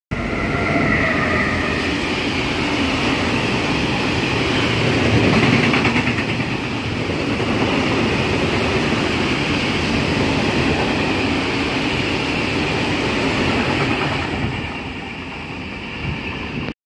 Sichtung aus Neumarkt: Vectron 193 756-4 ELL "750ste Vectron" mit Kesselwagen Richtung Passau.
Aufgenommen 09.2023.